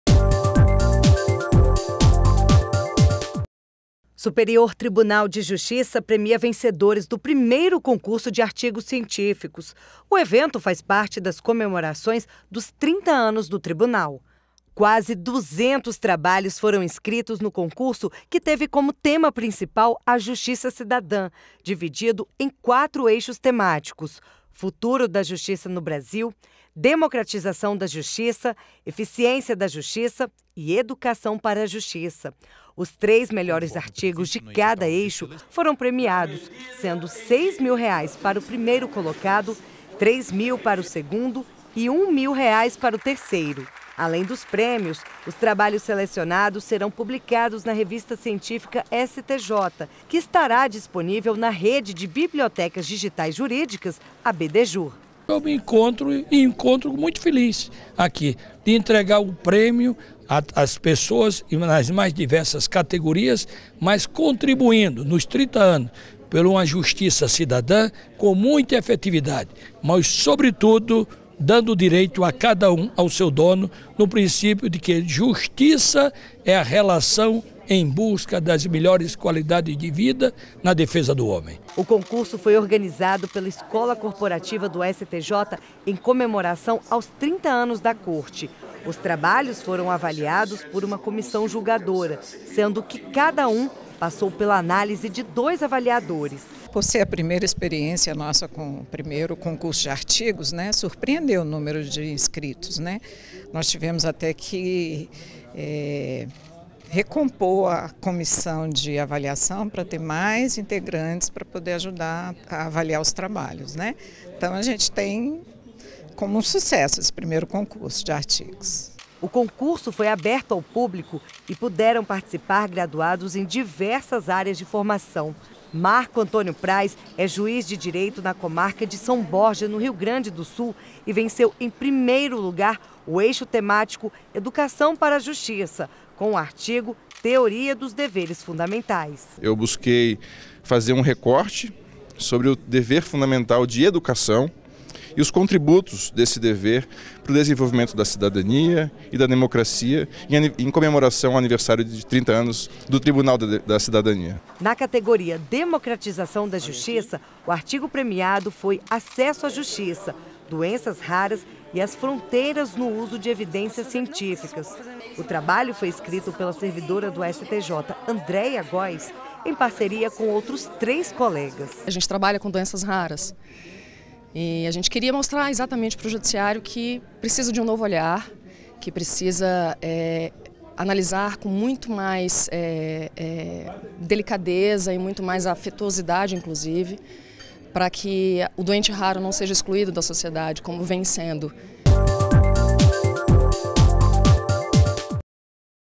Superior Tribunal de Justiça premia vencedores do primeiro concurso de artigos científicos (30.09)
Quase 200 trabalhos foram inscritos no concurso que teve como tema principal a Justiça Cidadã, dividido em quatro eixos temáticos: Futuro da Justiça no Brasil; Democratização da Justiça; Eficiência da Justiça e Educação para a Justiça.